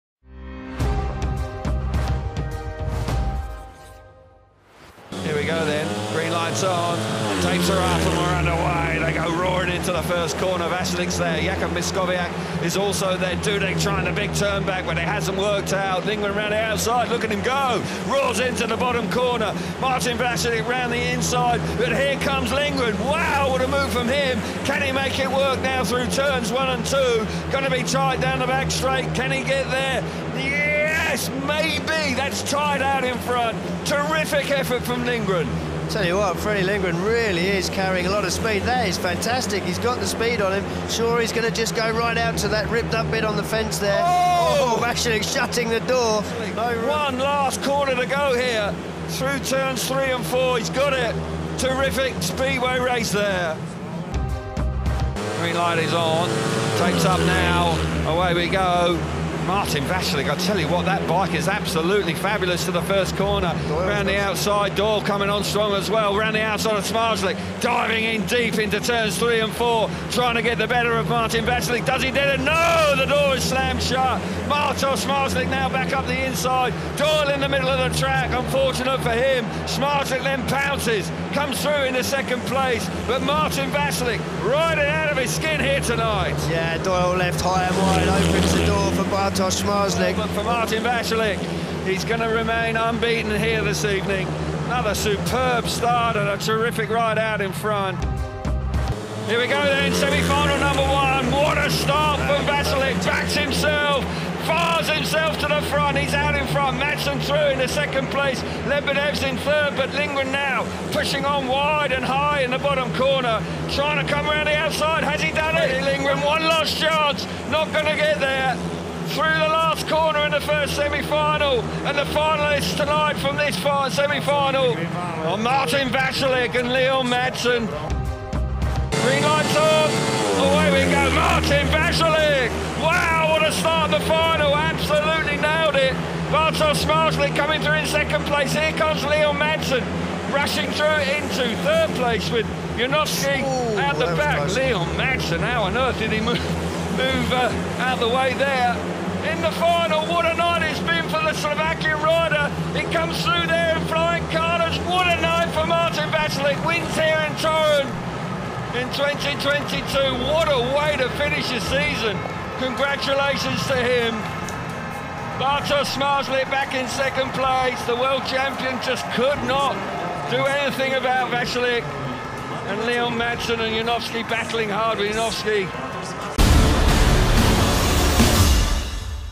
Martin Vaculik Rider of the Week | FIM Speedway Grand Prix
Winning his second Grand Prix of 2022 and scoring an impressive 19 points from seven rides, Martin Vaculik is our top rider from last Saturday's